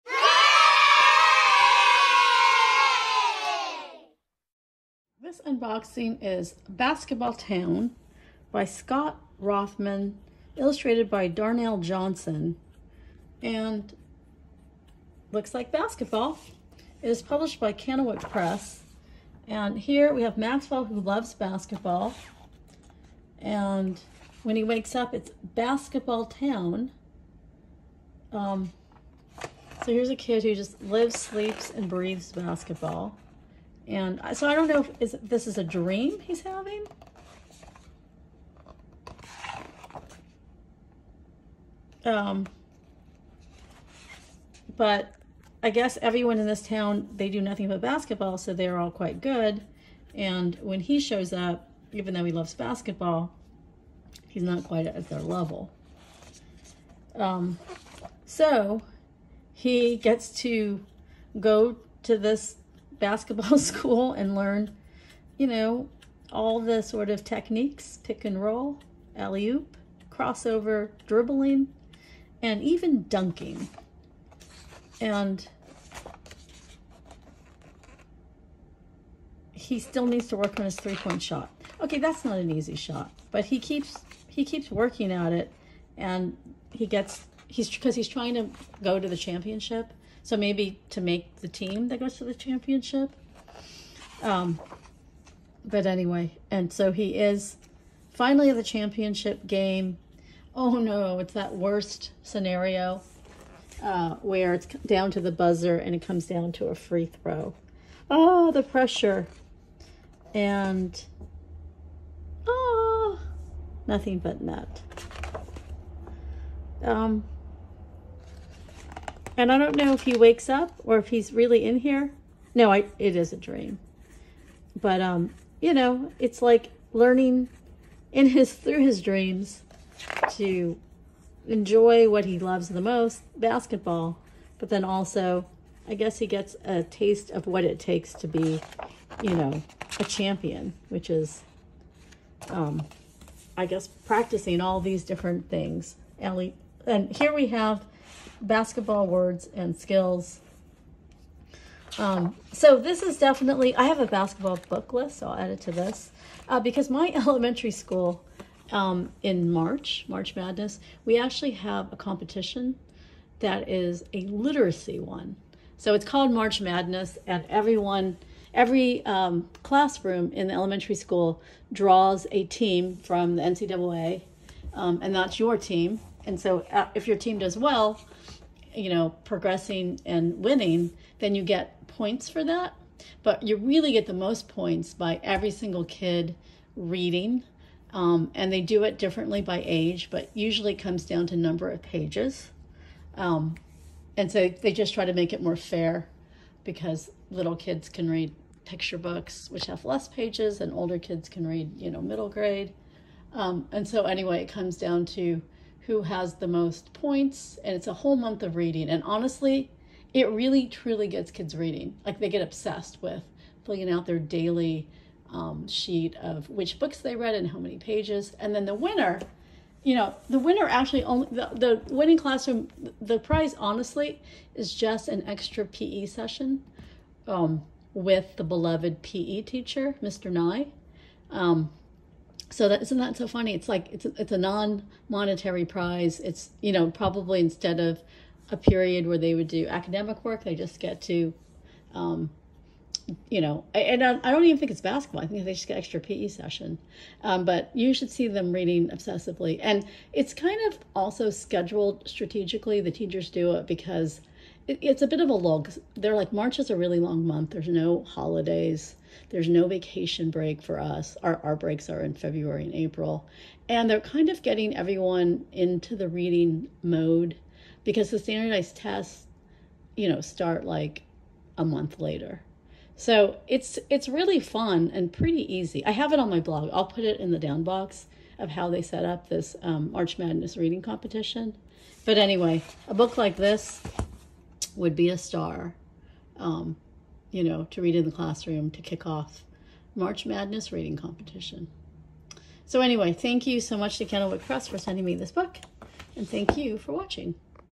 Unboxing// Basketball Town by Scott Rothman
Unboxing// Basketball Town by Scott Rothman, illustrated by Darnell Johnson

Published by Candlewick Press

Here's my post on my elementary school's March Madness Basketball Reading Program ...

Here's my March Madness: Basketball Books for Every Kid (ages 4 and up)!: